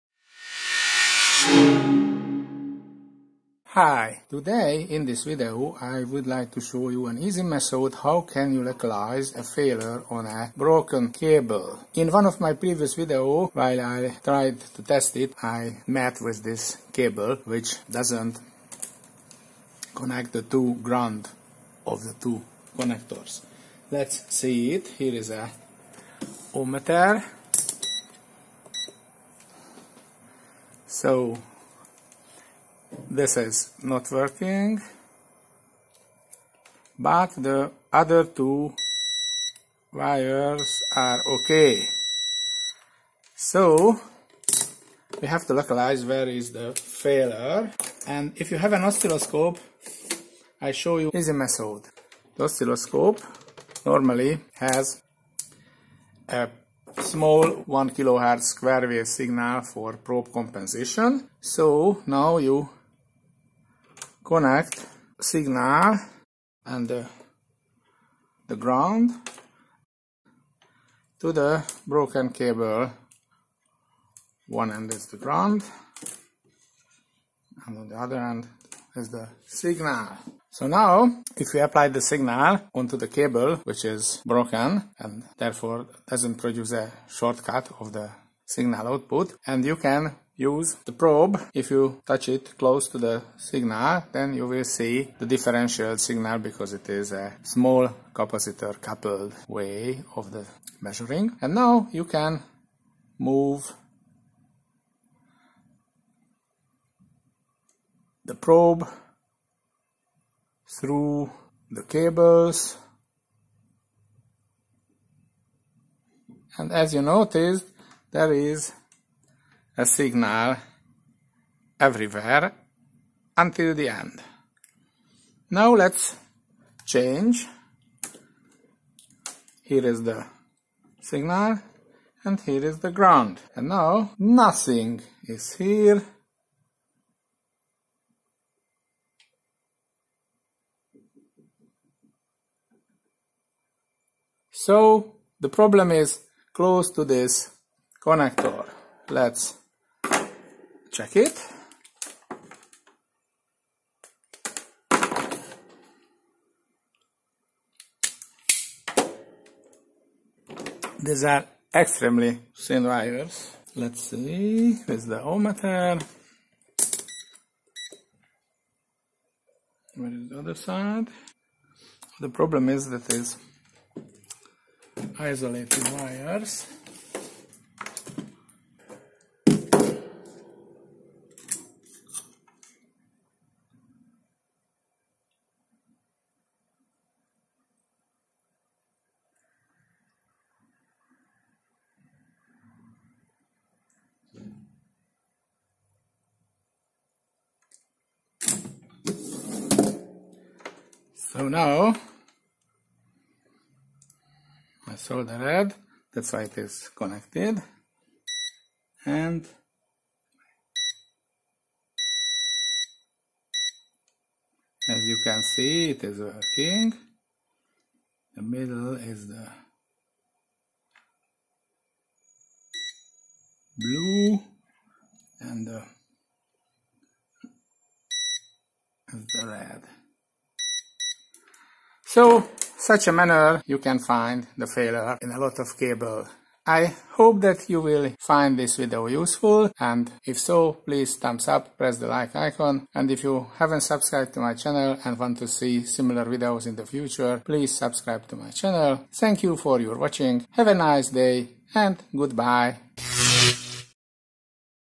How to find a break in a cable or a wire
In this video I show you an easy method to find a break in a wire or in a non shielded cable with a use of an oscilloscope.